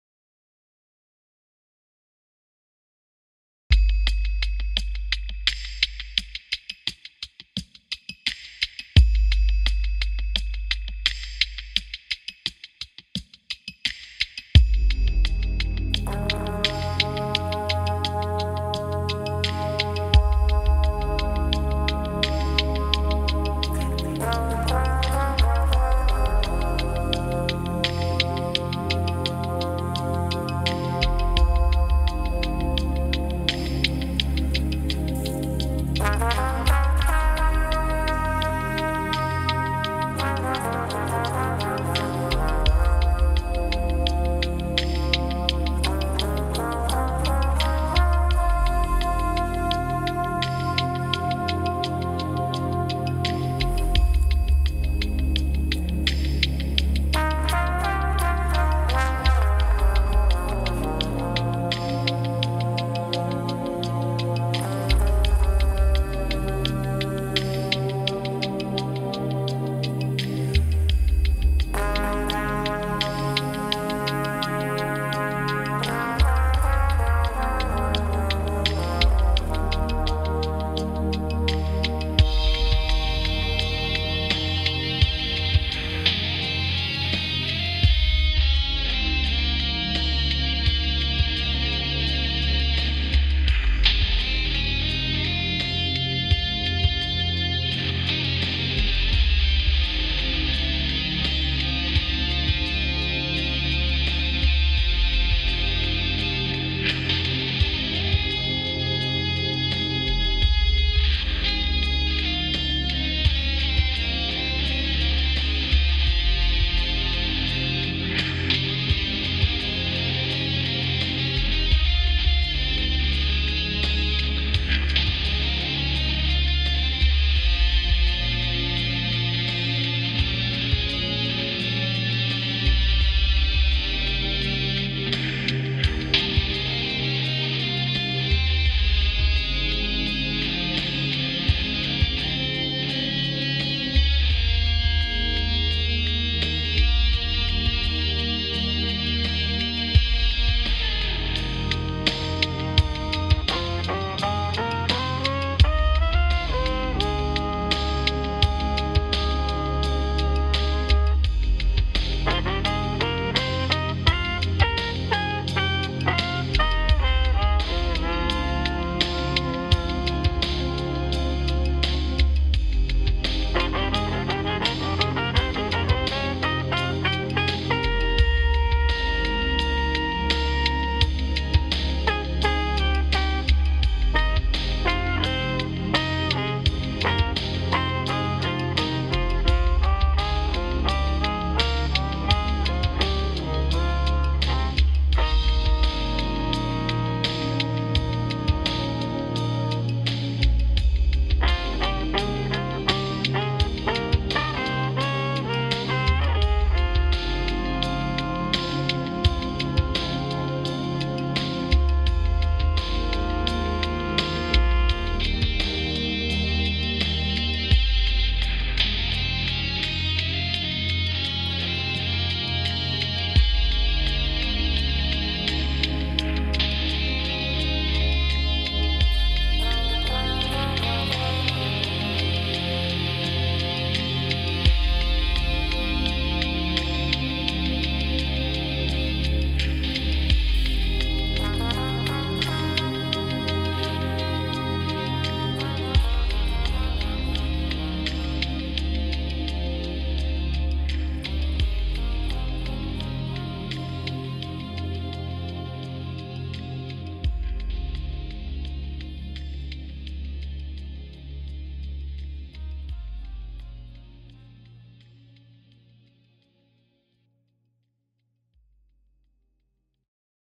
Firehawk FX Trombone
Trombone experiments through a Line6 Firehawk FX effect board.
First 3 effects recorded with a clip microphone on a valve trombone.